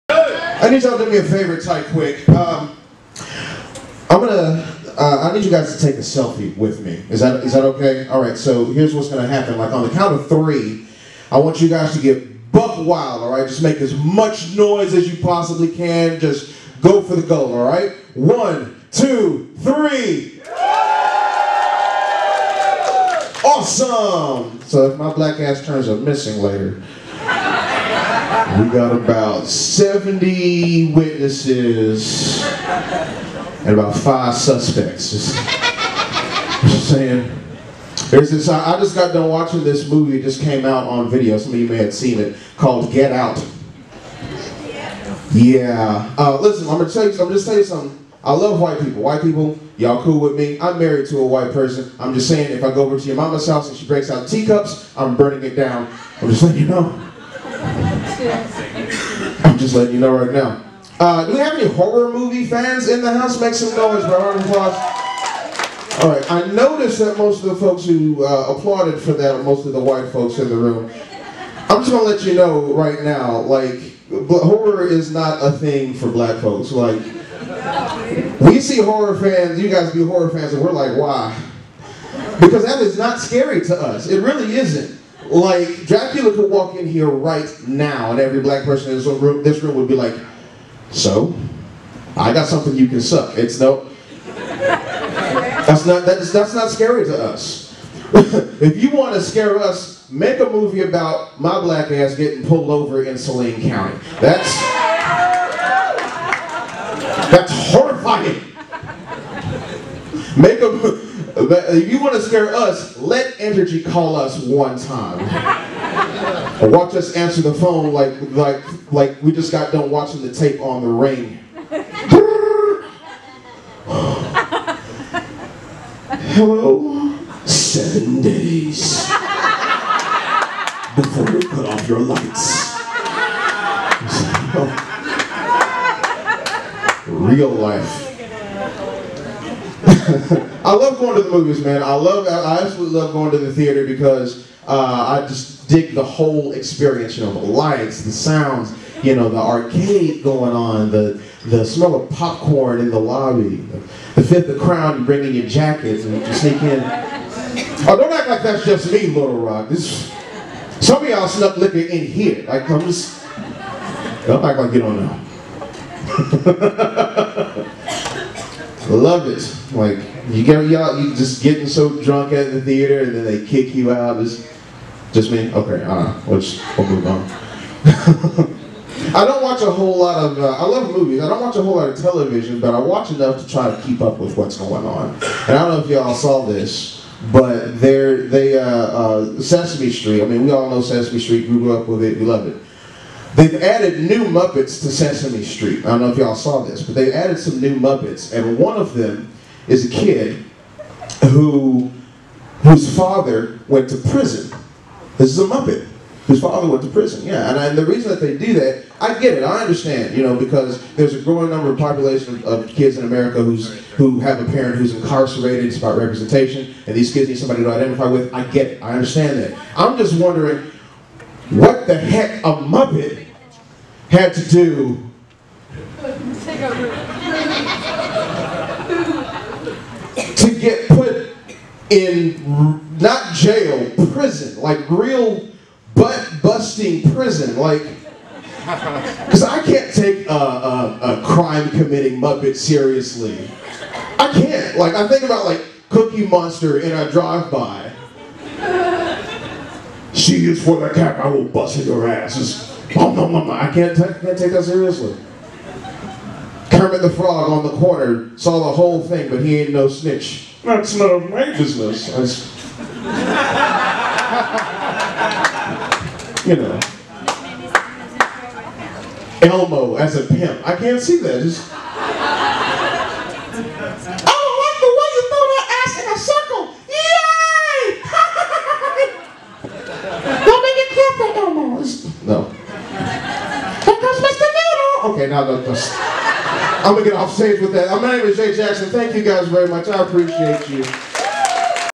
Jay Jackson - Standup at the Loony Bin IV
Another set at my home club in Little Rock: this time, doing a guest spot with Dan Fritcshie, Keith Terry, and Joe Lowers. Always a great time at the Loony Bin!